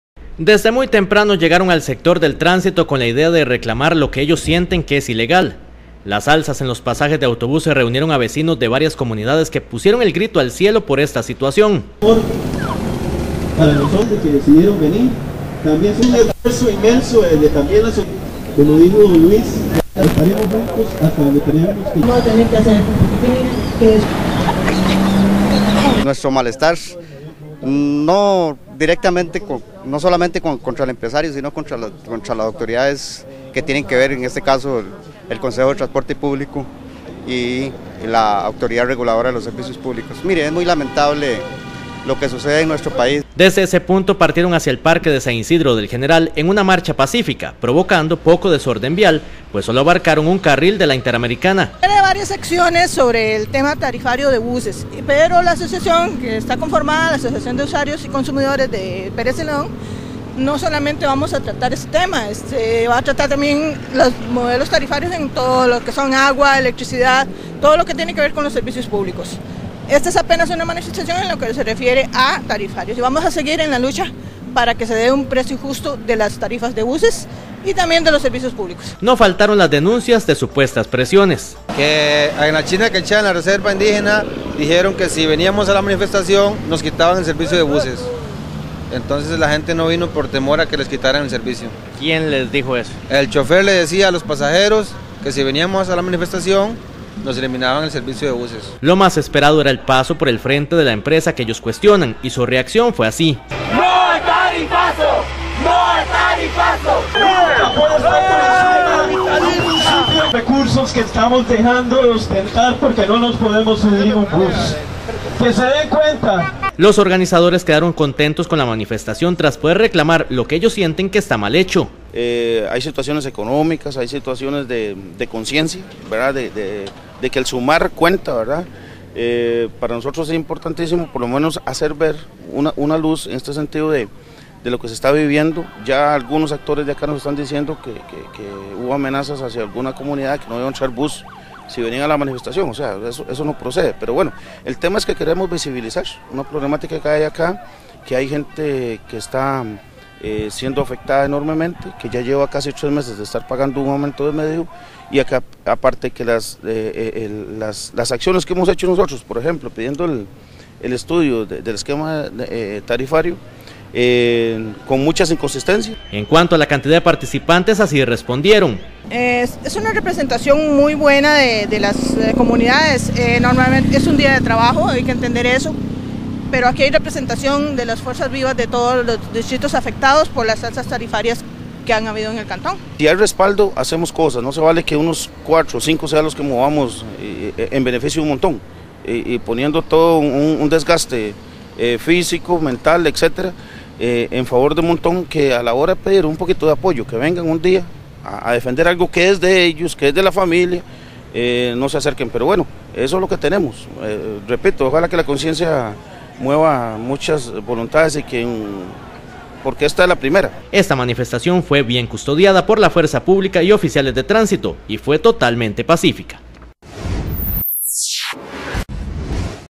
CIUDADANOS SE MANIFESTARON ESTE VIERNES EN CONTRA DE LAS ALZAS EN LOS PASAJES DE AUTOBUSES
Desde muy temprano llegaron al sector del transito con la idea de reclamar lo que ellos sienten que es ilegal, las alzas en los pasajes de autobus reunieron a vecinos de varias comunidades que pusieron el grito al cielo por esta situacion